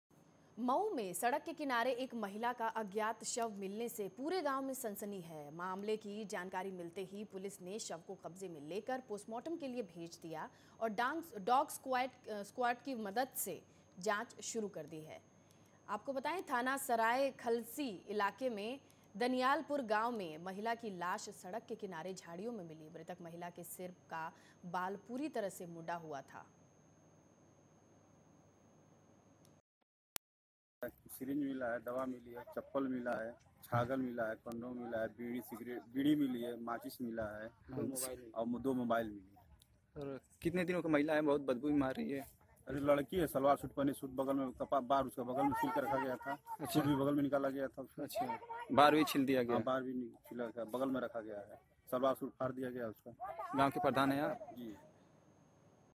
National Voice : Mau Police found woman's dead body
Mau police found unidentified  woman's dead body.